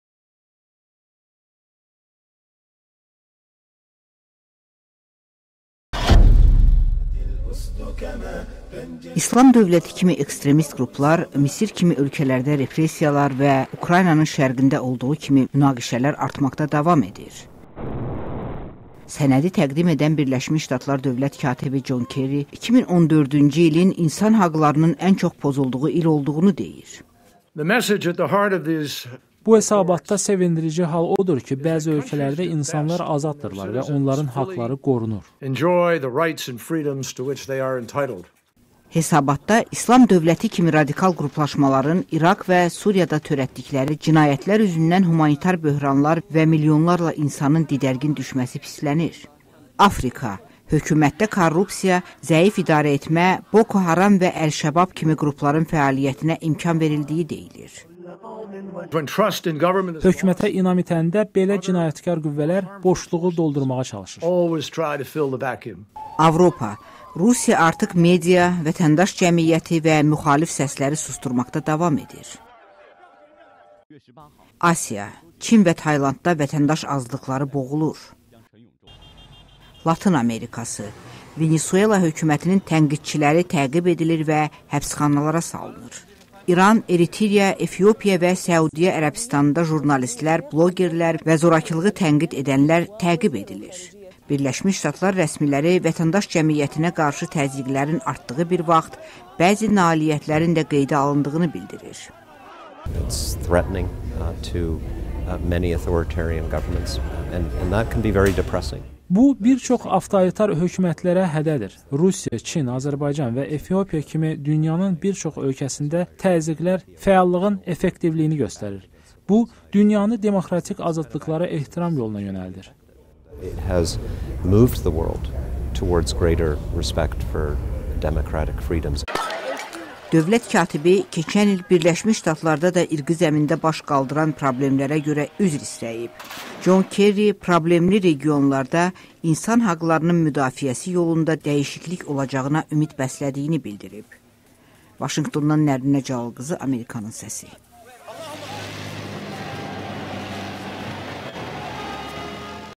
2014-cü il hesabatı - Dünyada insan haqları
ABŞ hökumətinin açıqladığı hesabatında diqqət dünyadakı terrorçu qruplaşmalara, korrupsiyaya və yoxsul dövlətlərdəki problemlərinə yönəlib